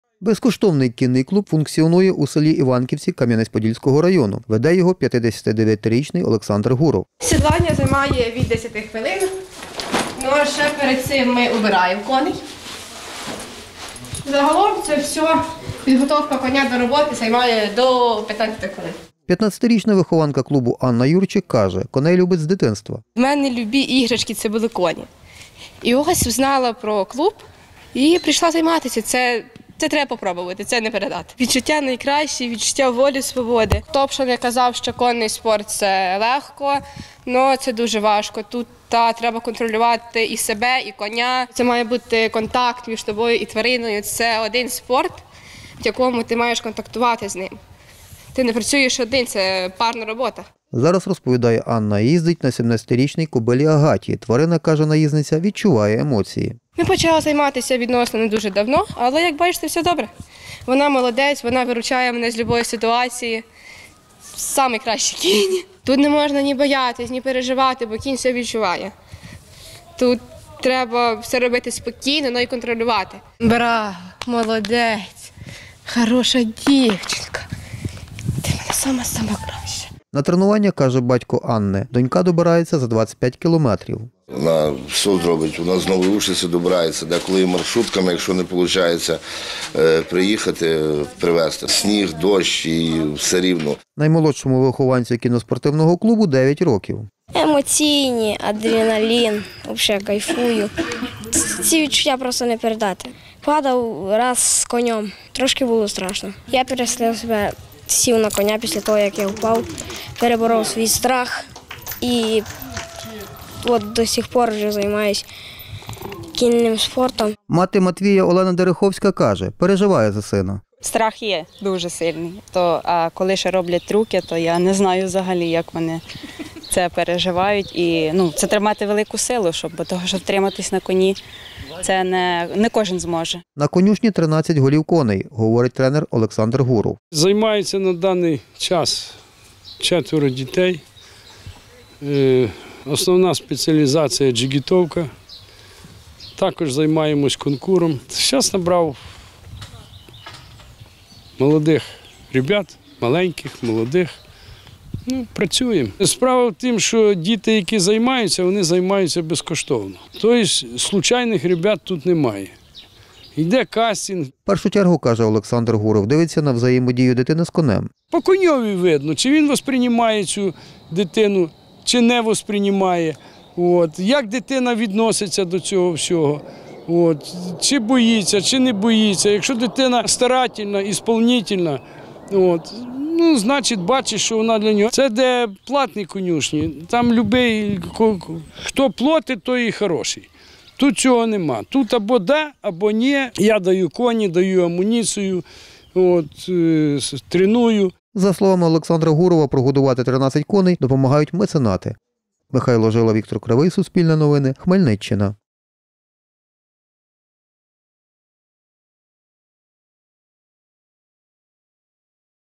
На Хмельниччині діє безкоштовний кінний клуб
Безкоштовний кінний клуб веде 59-річний Олександр Гуров з села Іванківці Кам’янець-Подільського району. Наймолодшому підопічному майстра з кінного спорту 9 років. За словами Олександра Гурова, «Кінно-спортивний клуб» два місяці тому здійснив новий набір бажаючих займатися кінним спортом. Тож хто займається у клубі, які трюки виконують та які враження отримують від спілкування з тваринами далі у сюжеті.